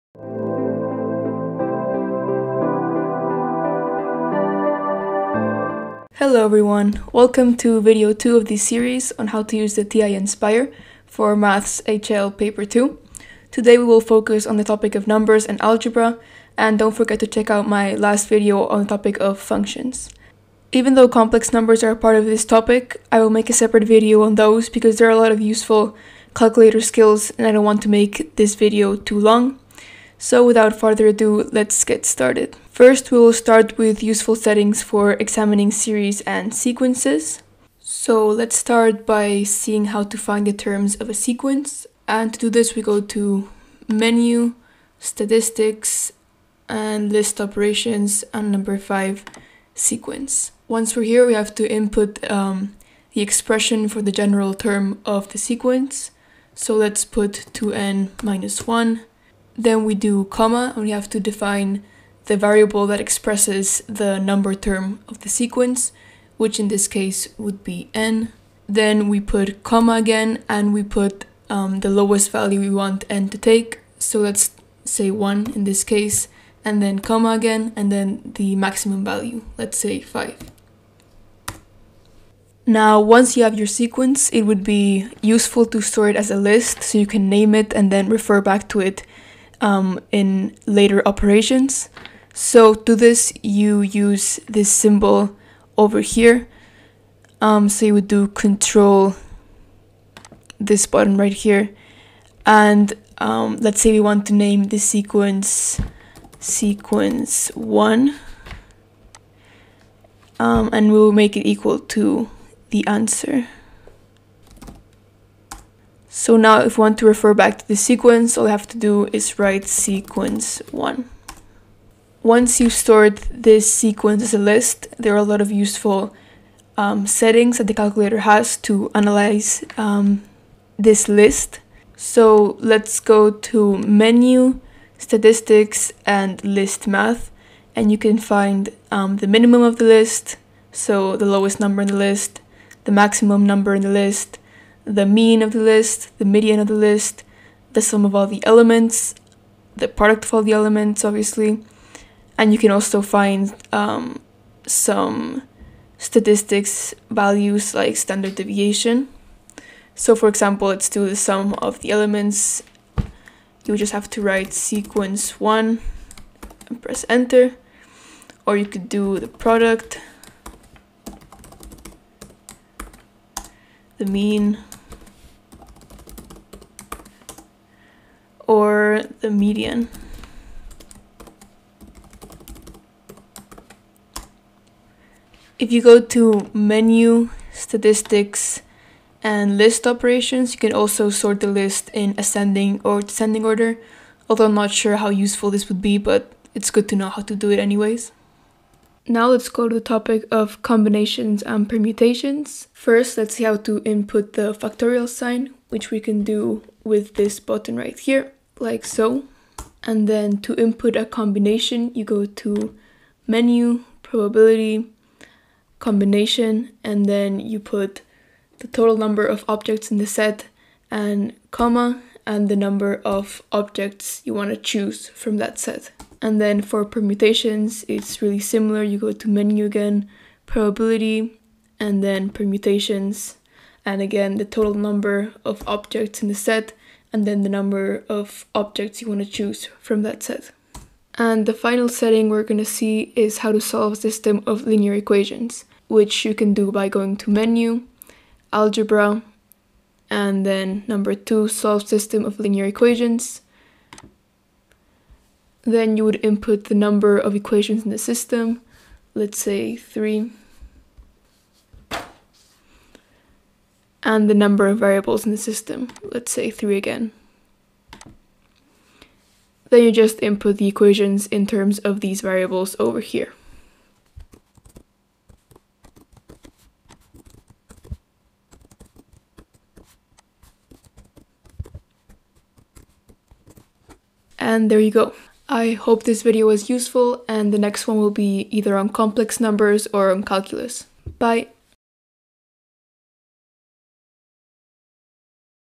IB Math AA HL - How to use your TI-nspire for Paper 2 (Numbers & Algebra)
In this video, I will be going through the calculator skills that I learned for my May 2021 IB Math Analysis and Approaches HL paper 2, specifically those having to do with the topic of Numbers and Algebra (minus complex numbers).
Timestamps:
0:37 - How to find the terms of a sequence
1:22 - How to store your sequence as a list with a name
2:01 - Useful settings to analyze sequences
3:18 - Settings for combinations and permutations
4:01 - Solving systems of linear equations
I intend to do more of these calculator videos for calculus, statistics, probability, etc.
Let me know if there are other Math/IB-related videos you would like to see!